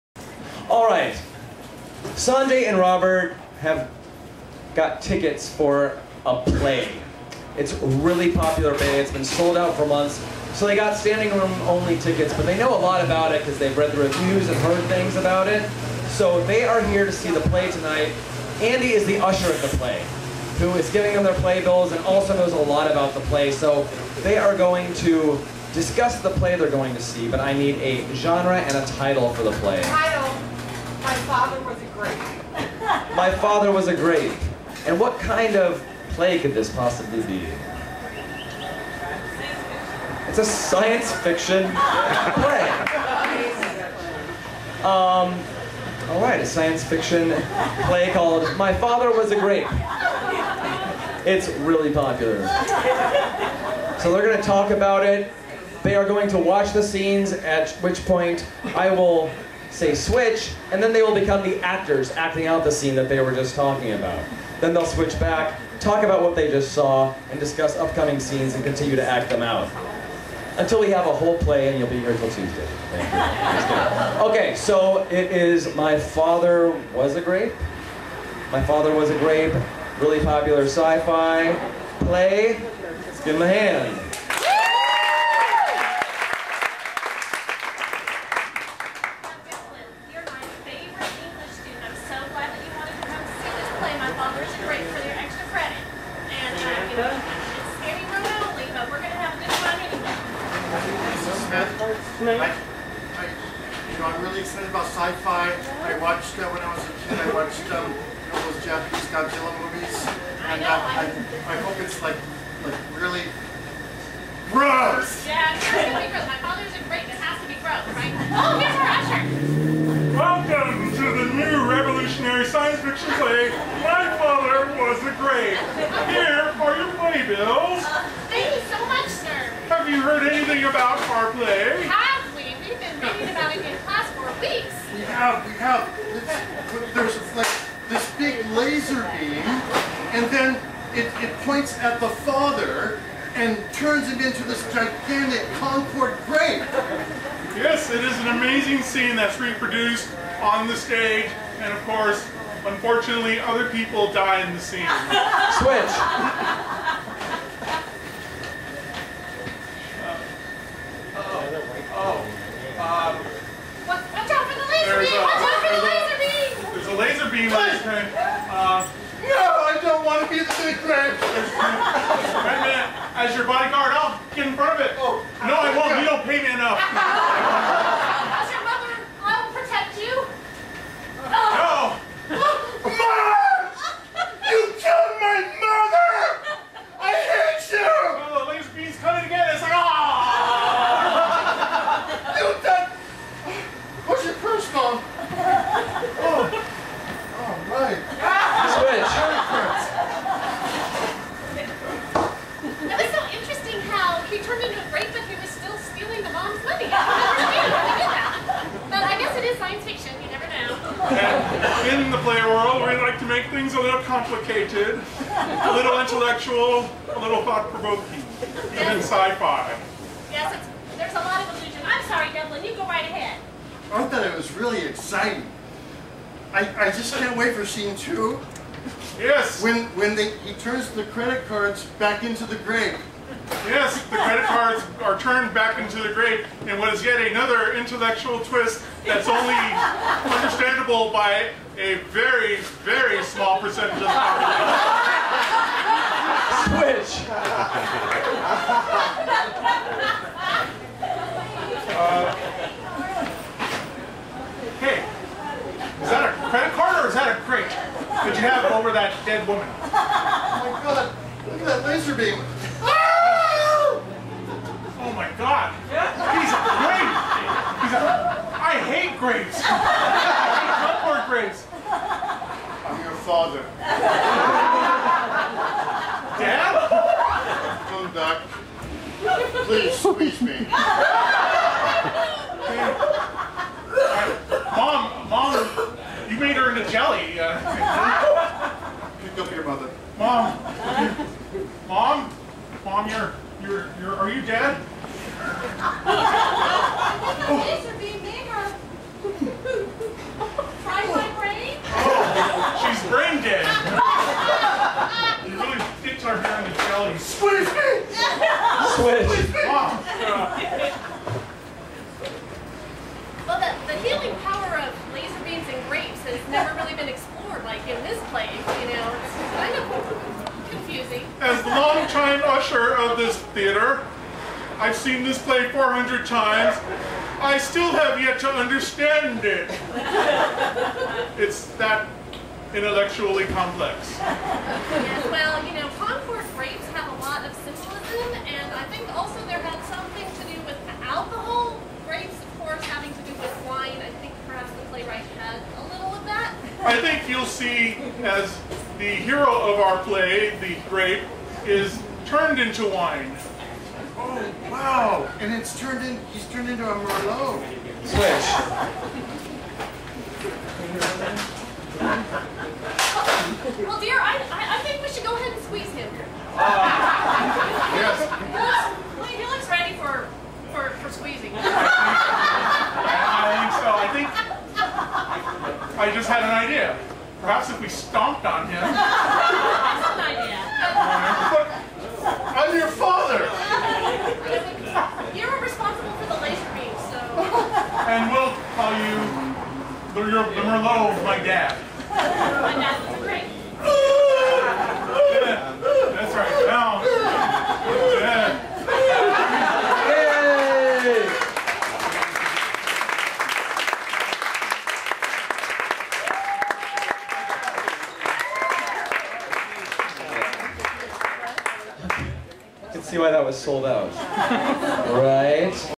My Father Was a Grape
Everyone enjoys a night at the theater, especially when it's the science fiction play, "My Father Was a Grape."  This action packed play will leave you on the edge of your seat -- twists and turns in every scene.  By the end, you'll be whining to see more.